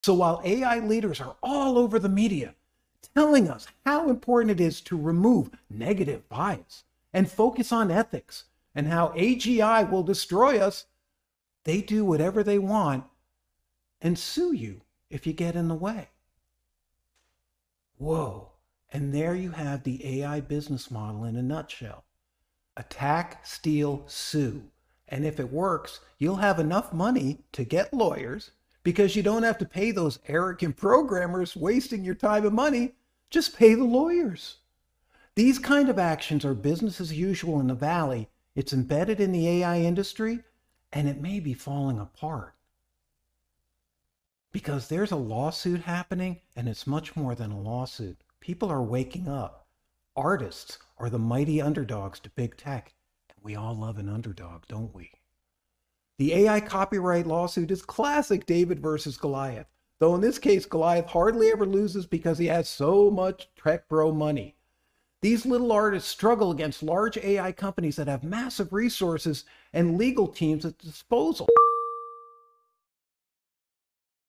The AI Business Model - Attack. Steal. Sue....and then say anything on the Internet is fair game
AI leaders preach about ethics and stopping bad bias, then do things that aren't even close to ethical.

- The Schmidt Hits the Fan - Private recording released

- Billionaires and Unicorns versus Artists - who wins?

- The Big Tech Goliath Bias – Act Fast and Sue People who get in your way

- The Artists are the Mighty Underdog against AI

- One Small Legal Victory, One Giant Challenge to the AI Industry

- Maybe ethics Matter. will AI do the right thing?